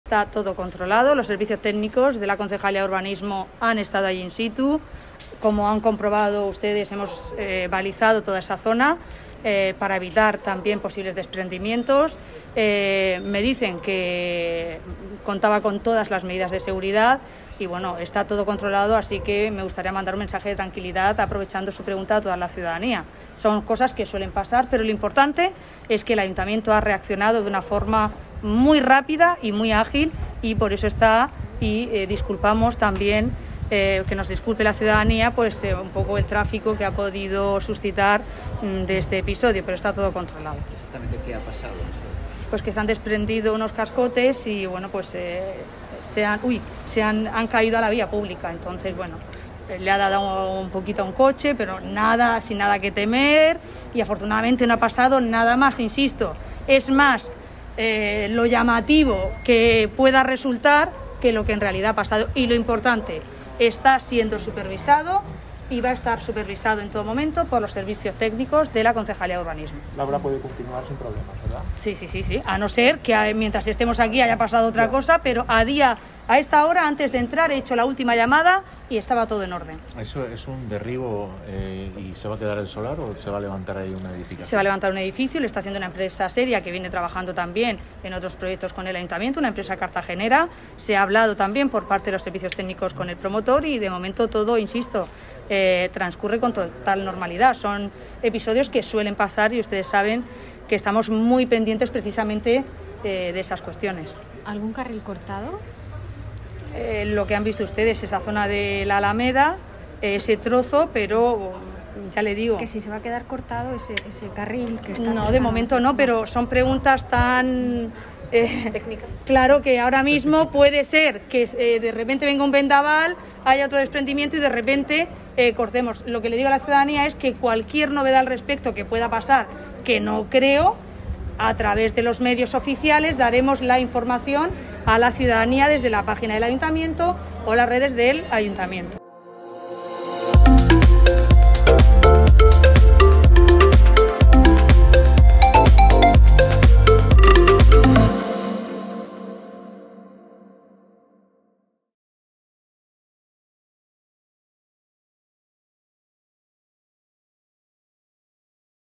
Urbanismo da por controlada la caída de cascotes provocada por la demolición de un edificio
Las obras de demolición de un edificio situado en la Alameda de San Antón, junto a la Plaza de España, se han topado en la mañana de este viernes 22 de octubre con un problema al tirar un muro medianero con el edificio colindante, que ha provocado la caída de cascotes al solar y el desplazamiento de piedras hacia la acera y una parte de la calzada tras arrastrar la valla de protección.

Hasta el lugar se han dirigido inmediatamente los Servicios Técnicos de la Concejalía de Urbanismo donde, tal y como ha explicado al vicealcaldesa y edil del área, Ana Belén Castejón, “han balizado la zona y comprobado que se cumplían todas las medidas de seguridad, con lo cual podemos asegurar que en este momento está todo controlado”.

Castejón ha querido hacer un llamamiento a la tranquilidad de la ciudadanía ya que, a pesar de que una parte de los cascotes ha llegado a la vía pública, afectando al tráfico, concretamente a un vehículo que estaba en el semáforo,” no ha habido que lamentar daños personales”.

La actuación que se está llevando a cabo para levantar un edificio en esta parcela es una obra con licencia de demolición, con su dirección técnica y coordinador de seguridad y salud.